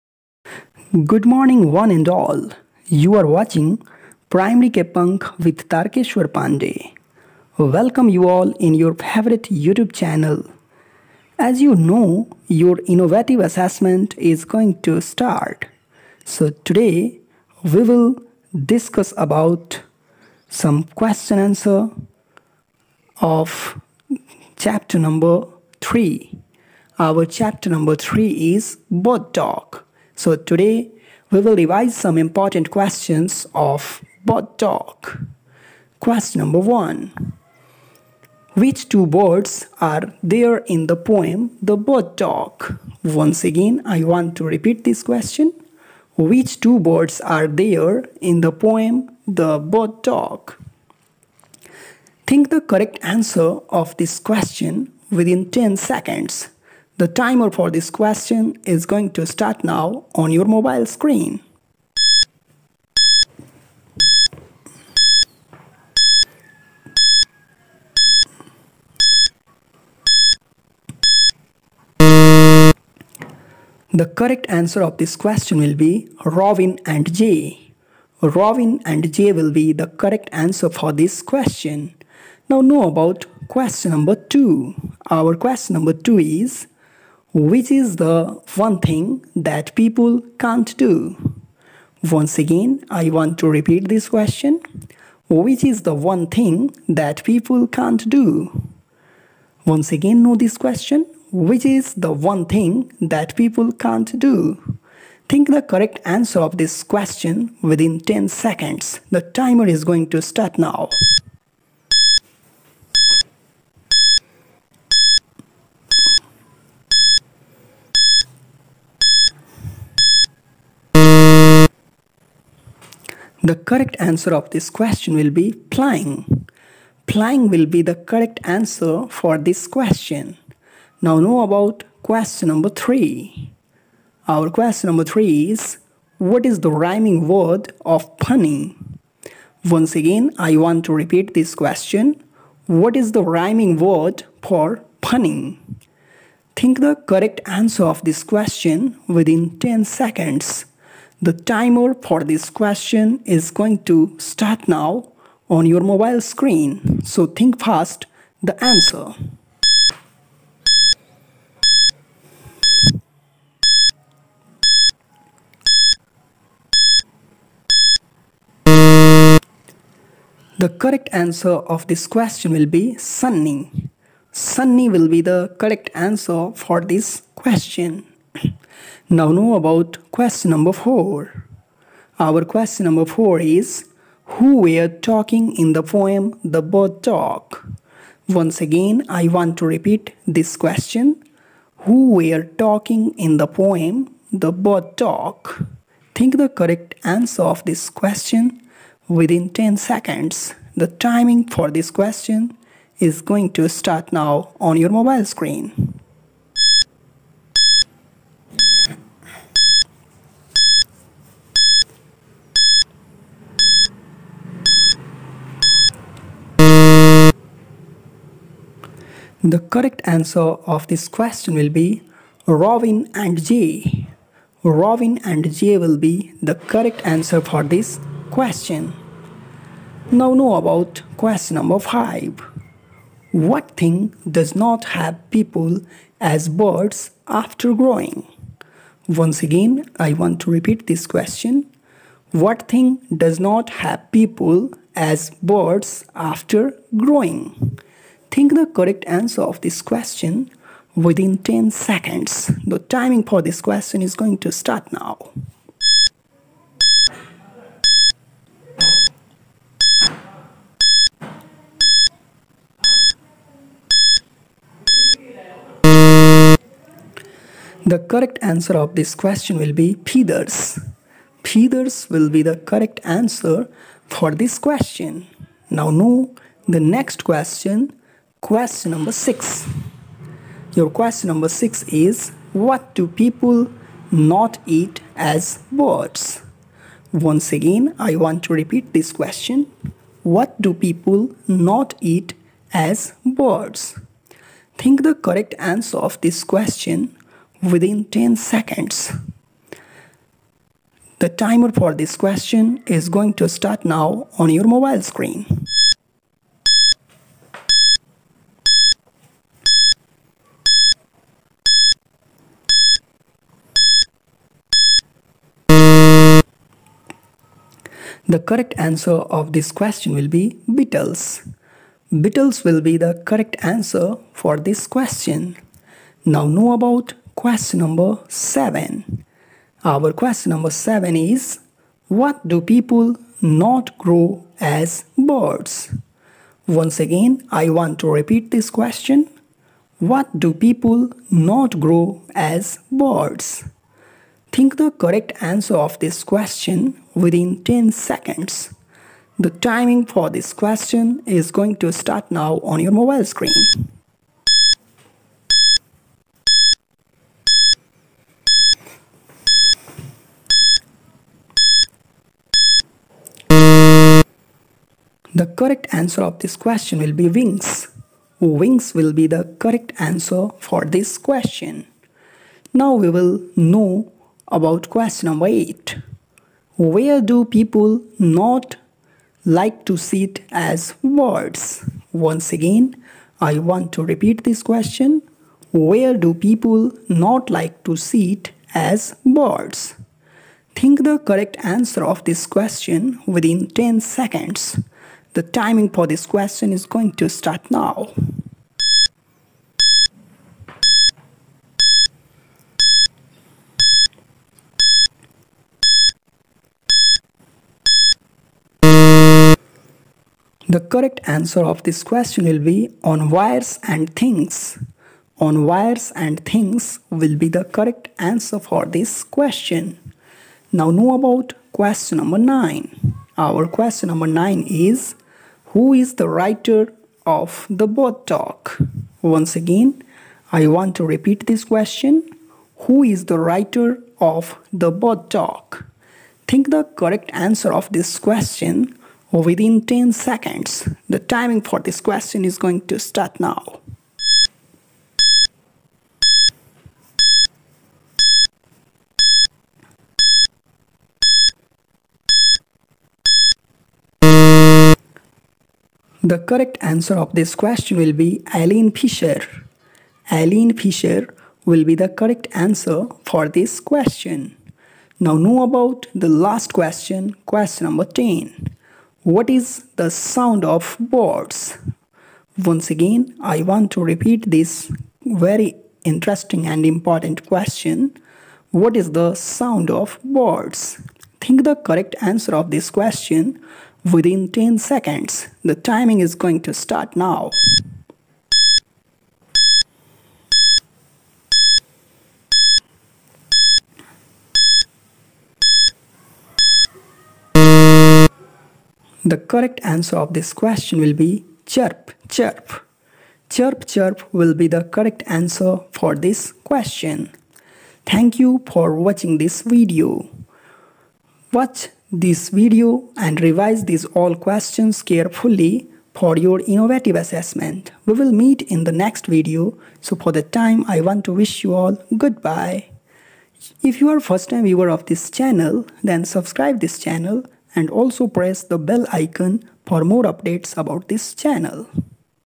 Innovative Assessment (Bird Talk) Chapter-3 English Class-3 🐦 By: Tarkeshwar Pandey
Innovative Assessment
Bird 🐦 Talk 🐦
Bird Talk
English Class-3
Chapter-3
By: Tarkeshwar Pandey
We will also support you with different knowledge...